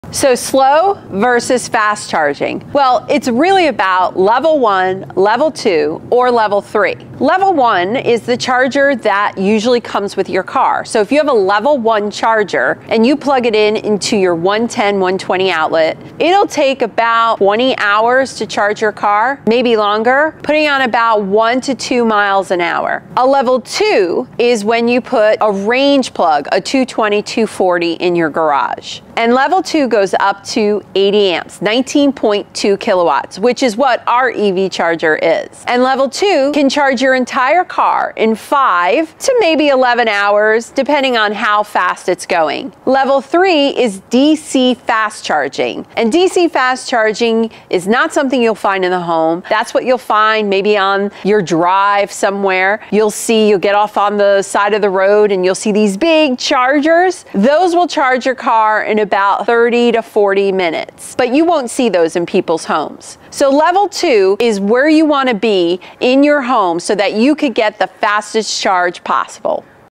EV Charging Explained: Level 1 vs. Level 2 vs. Level 3 – Which is Best for You?
As the adoption of electric vehicles (EVs) continues to grow, it's important to understand the different charging levels available. Let's dive into the details:

Level One Charging: The standard charger that comes with your EV can be plugged into a regular 110/120V outlet. However, this method is quite slow, providing only 1-2 miles of range per hour of charging. It can take up to 20 hours to fully charge your vehicle.

Level Two Charging: This is the recommended option for home charging. By installing a 220/240V, 80-amp, 19.2 kW charger in your garage, you can significantly reduce the charging time. Level Two can charge your EV in 5-11 hours, depending on the specific model.

Level Three Charging: Also known as DC Fast Charging, this is the quickest option, but it's not something you'll find in a residential setting. These high-powered chargers, typically located at public charging stations, can charge your EV in just 30-40 minutes.

For the best charging experience at home, Level Two is the way to go. It provides a balance of convenience and efficiency, allowing you to charge your EV quickly and efficiently.

*Eligibility required to participate in this program.
Get a Free Charger & *Basic Installation